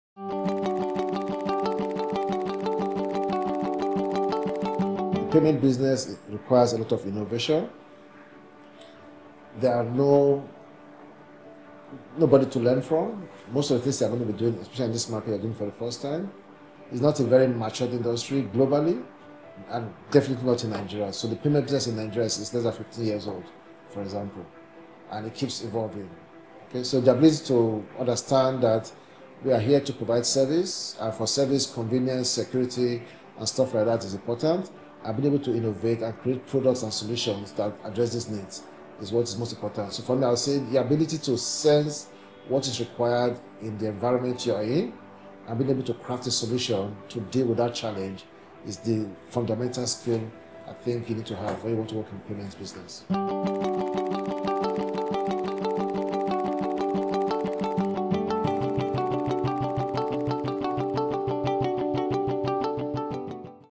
Career in Management...Mitchell Elegbe, Founder and CEO at Interswitch
Interswitch provides technology integration, transaction processing and payment infrastructure to government, banks and corporate clients. In recent years, major players in the payment industry has demonstrated great interest in the African market. Facing fierce competition, Interswitch continues to explore ways to improve the convenience and security factors in payment and transaction, enabling clients and customers to exchange value in an intuitive fashion.